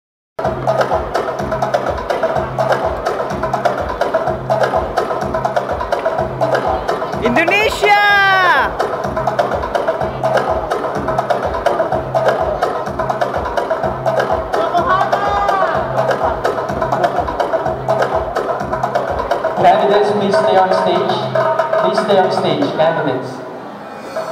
Man of the Globe International 2019 candidates in their dance of the Globe outfits
Man of the Globe International 2019
January 29, 2019,
SM Telebastagan, Angeles City, Pampanga